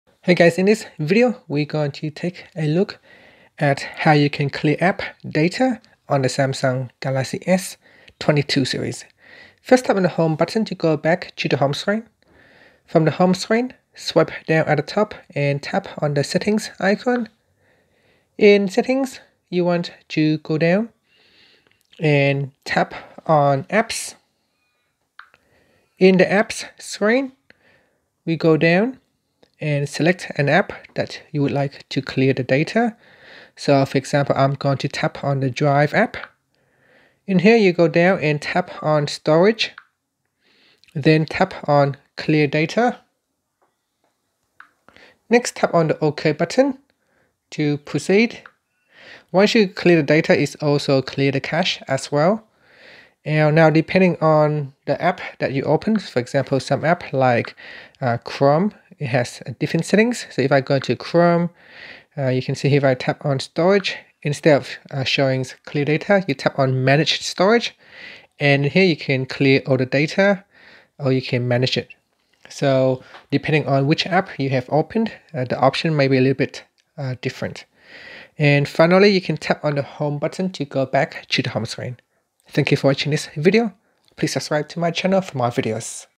Galaxy S22/S22+/Ultra: How to CLEAR APP DATA
Learn how you can CLEAR APP DATA on the Samsung Galaxy S22/S22+/Ultra.